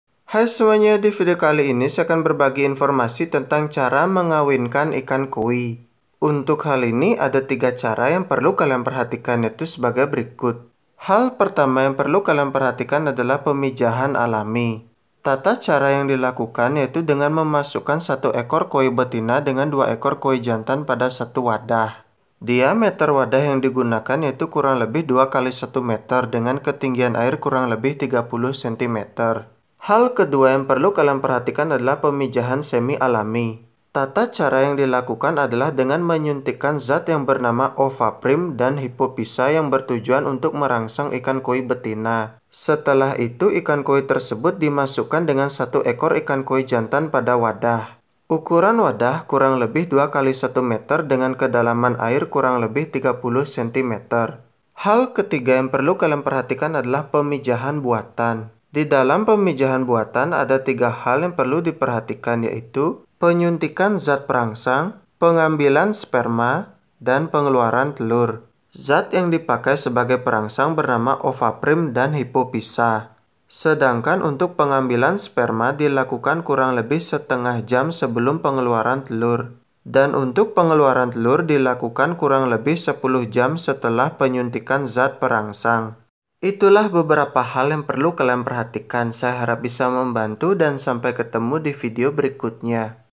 3 Cara PEMIJAHAN atau MENGAWINKAN ikan koi / How to BREEDING KOI FISH
Di dalam video ini dijelaskan tentag 3 cara mengembangbiakkan atau memijahkan ikan koi yang biasa dilakukan oleh para peternak ikan koi. saya harap setelah menonton video ini kalian merasa terbantu dan ilmu kalian juga bertambah.. TERIMA KASIH..